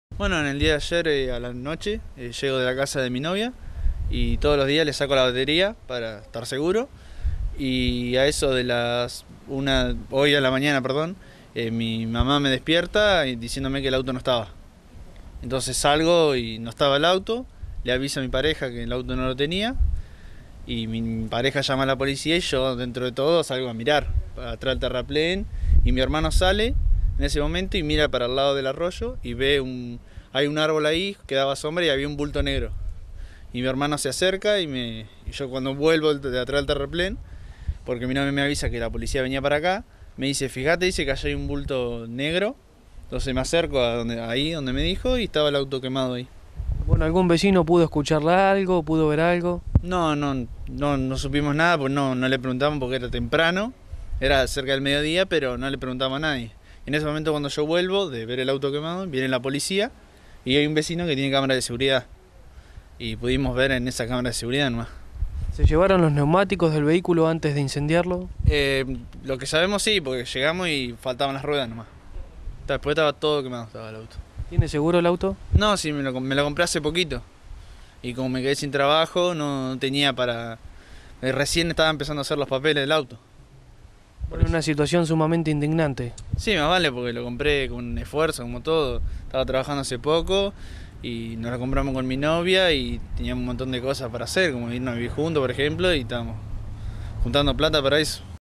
Robo e incendio de un vehículo
Federico Álvarez fue entrevistado por Noticias y describió cómo encontró su automóvil prendido fuego, luego de que fuera sustraído de la puerta de su domicilio.